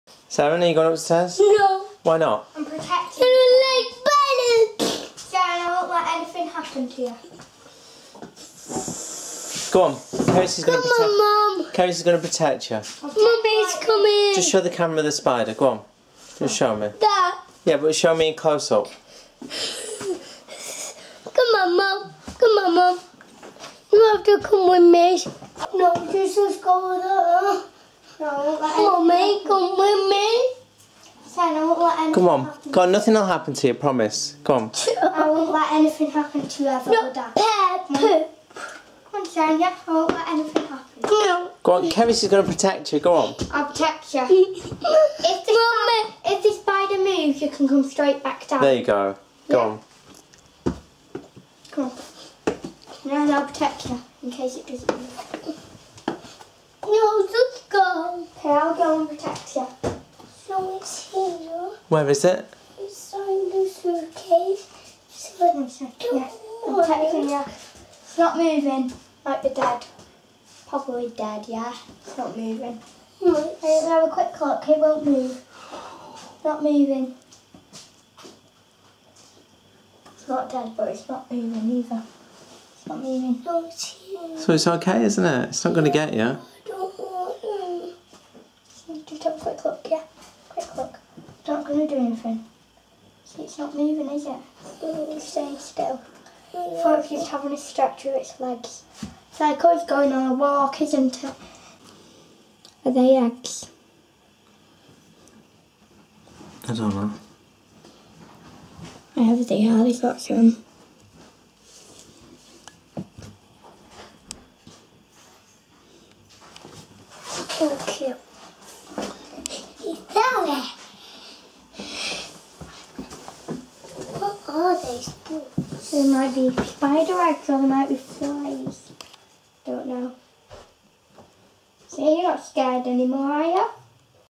Seren faces her fear of a spider. Big sister on hand to help.
I thought the best way of dealing with Seren's fear of the spider on the stairs was to go up to it & see there was nothing to be scared of. She got through it with Cerys' help.